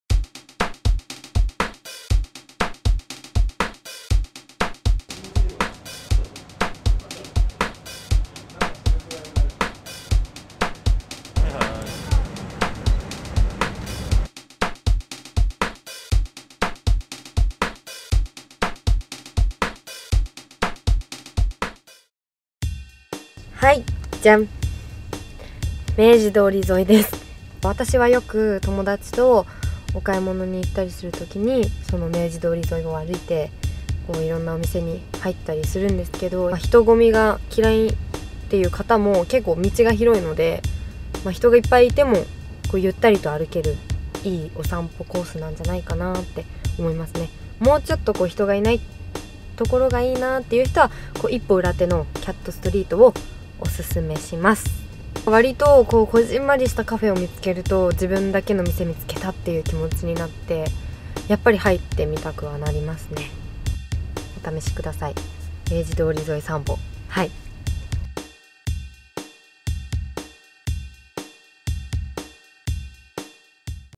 ノンノ５月号 新木優子のTOKYOオススメスポットは？
ノンノ５月号“大学４年生・優子のちょっとオトナなTOKYOおでかけ着回し10DAYS！”企画にちなんで、優子の個人的な東京オススメスポットを教えます♪